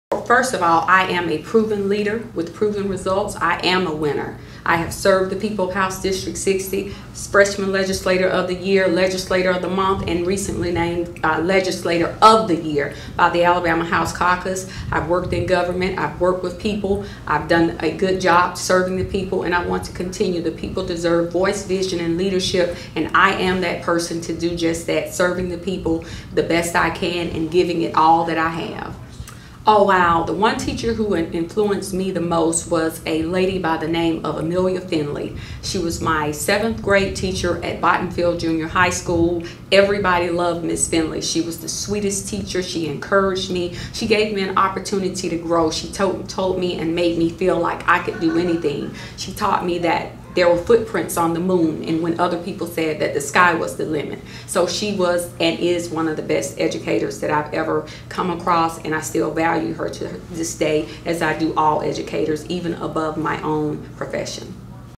Juandalynn Givan, candidate for HD60
Juandalynn Givan, Democratic candidate for Alabama House District 60, talks about why she is the best candidate, and what teacher influenced her the most.